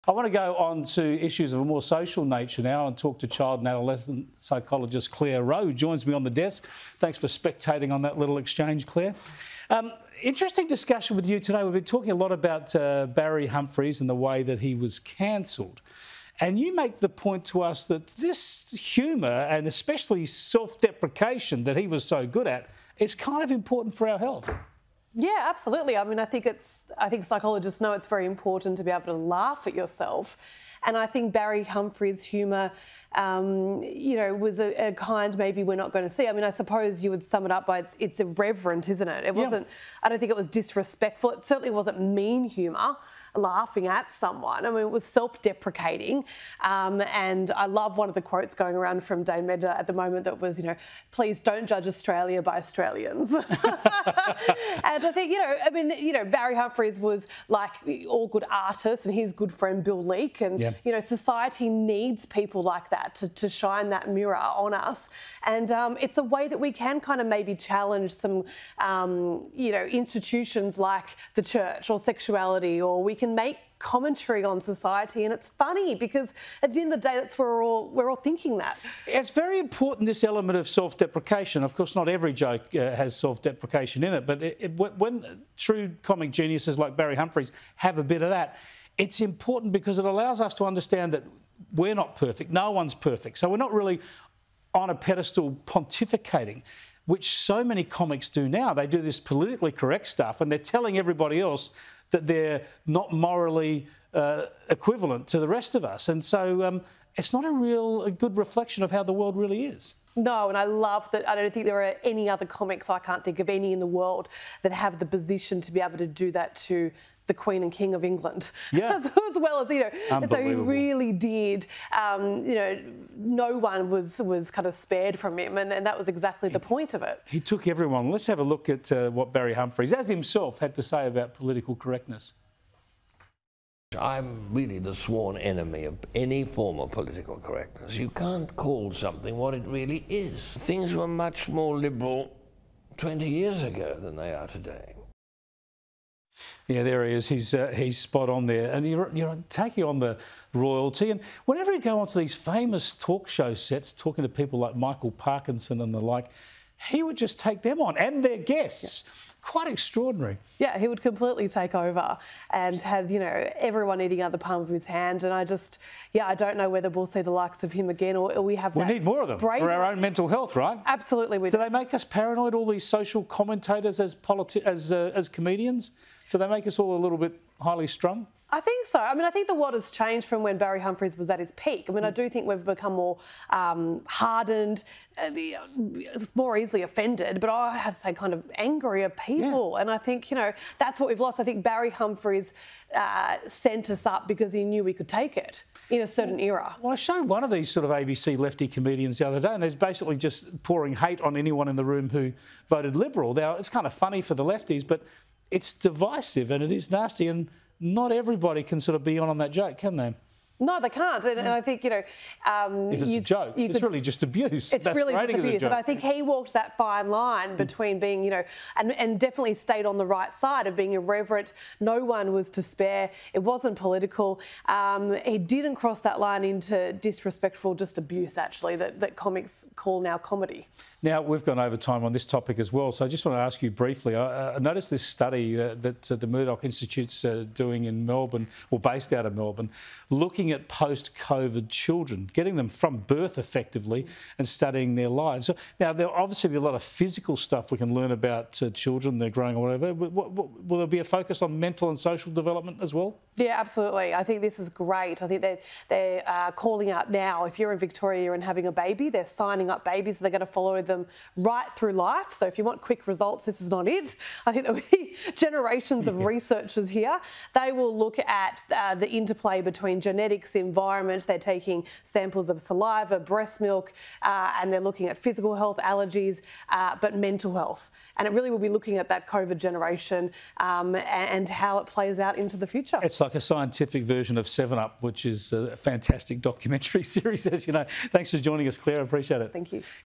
‘No one was spared from him’: The ‘irreverent’ humour of Barry Humphries
Child and Adolescent Psychologist Clare Rowe says “no one was spared” from the humour of Barry Humphries.

“He walked that fine line and definitely stayed on the right side of being irreverent,” Ms Rowe told Sky News host Chris Kenny.